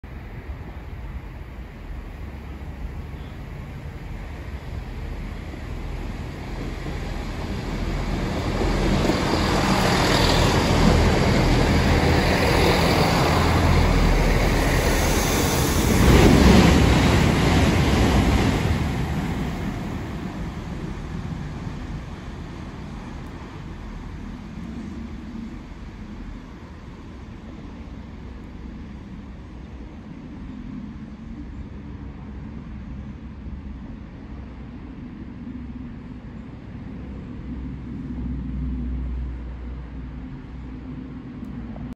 Class 166 Networker | 166220 | Great Western Railway | Tilehurst | 05/12/22
166220 passes Tilehurst on an ECS move.